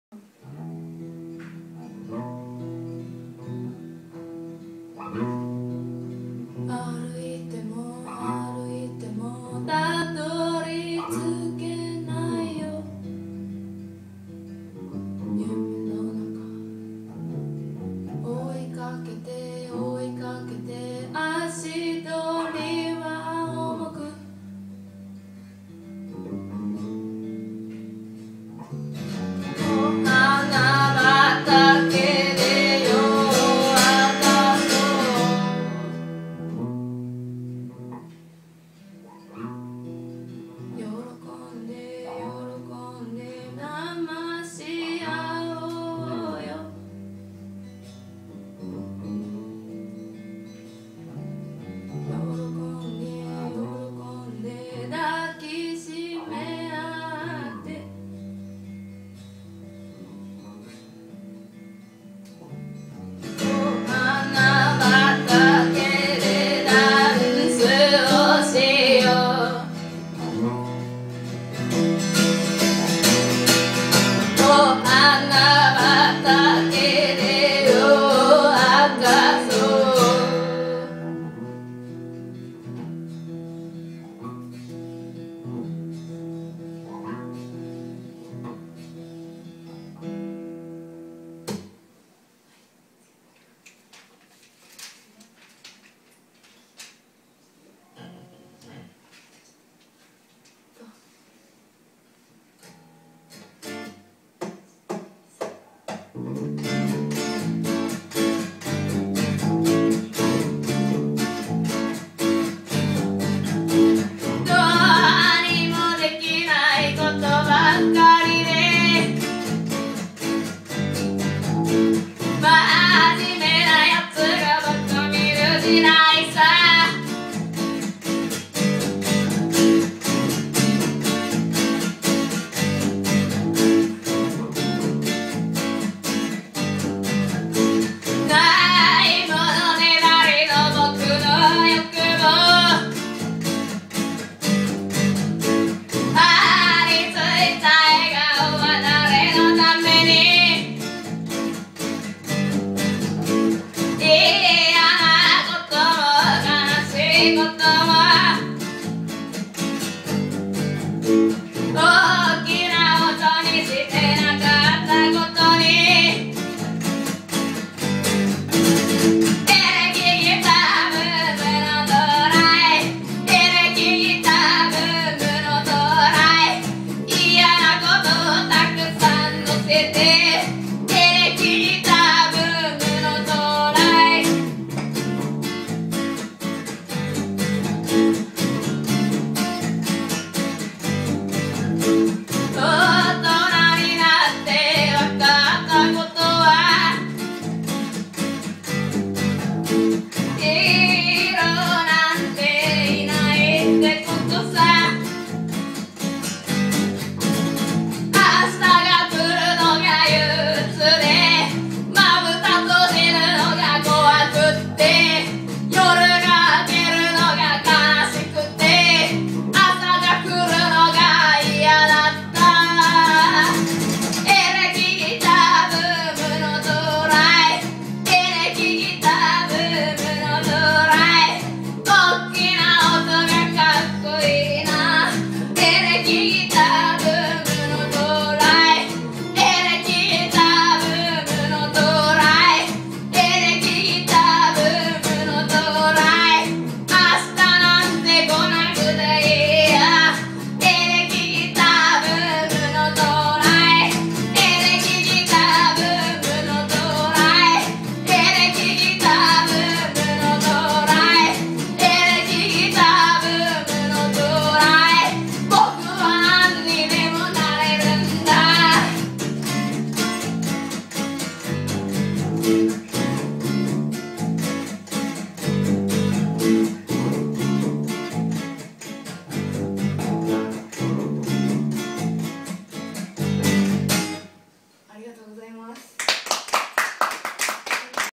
六箱 2013年2月21日@難波Mele
2013年2月21日 難波Meleにて行われたイベント「HAPPY TALK」でのライブ。